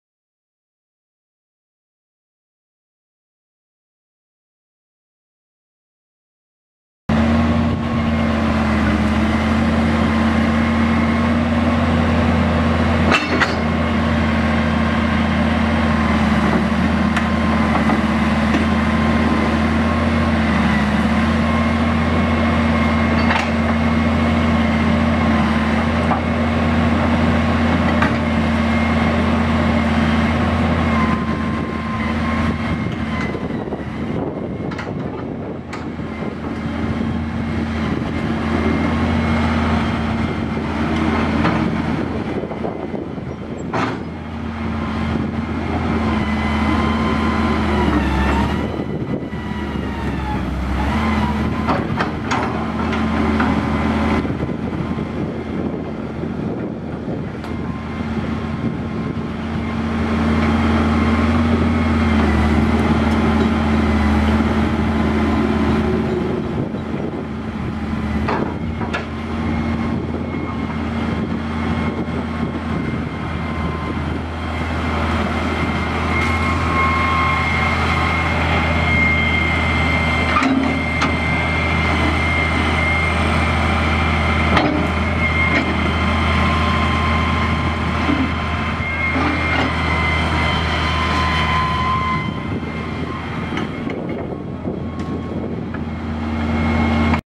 1986 Koehring 6625 excavator for sale | sold at auction July 31, 2014
1986 Koehring 6625 excavator
1,962 hours on meter
Six cylinder turbo diesel engine
9&#039; stick length
48"W bucket
30"W track pads
55,250 lbs. base operating weight
Serial 66497

It was located in Stanberry, MO, and we sold it to the highest bidder for $14,300.